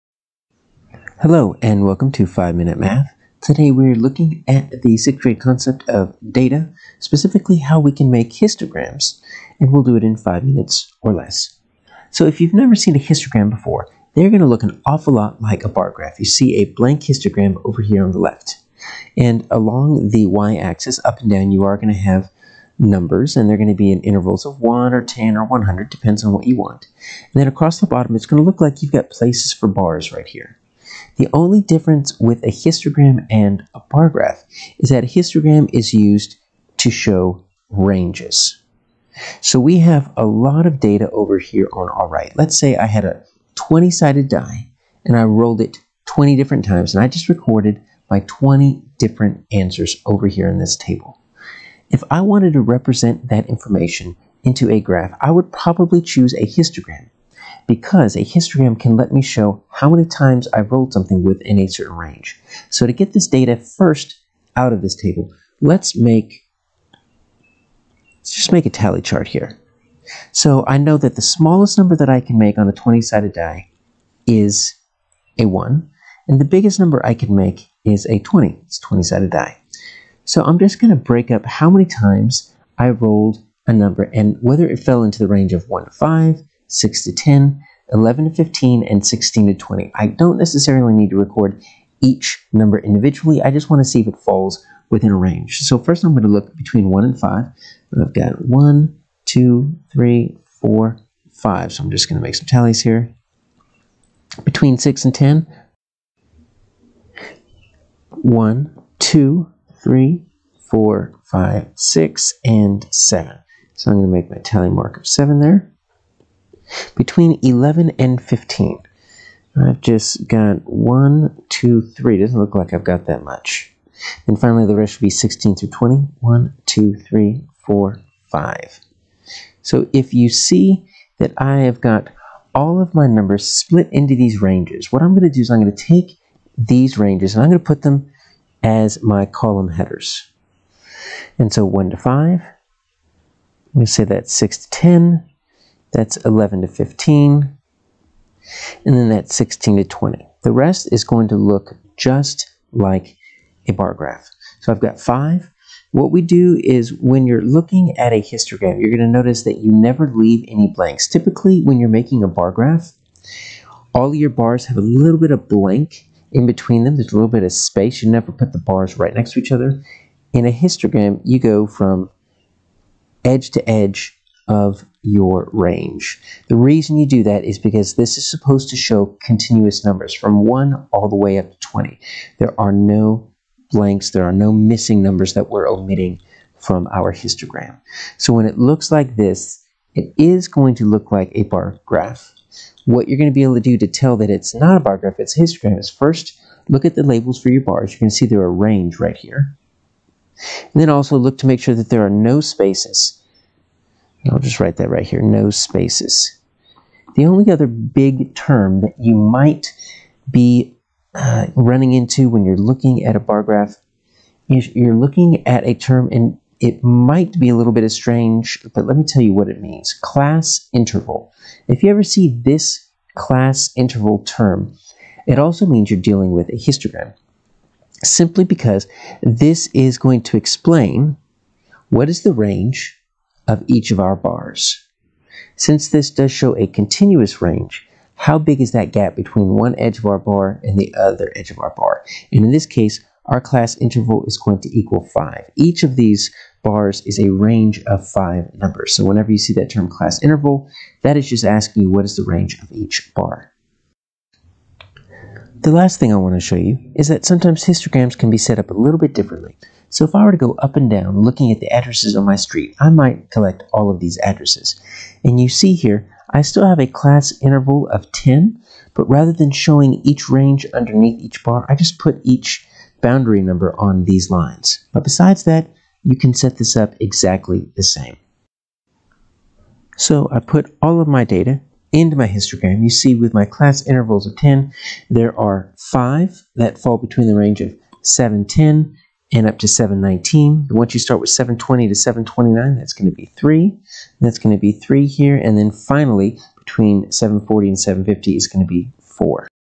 6th Grade Math Histograms
Learn how to create a histogram from a table of data, including how to find the class interval. This video contains two separate examples.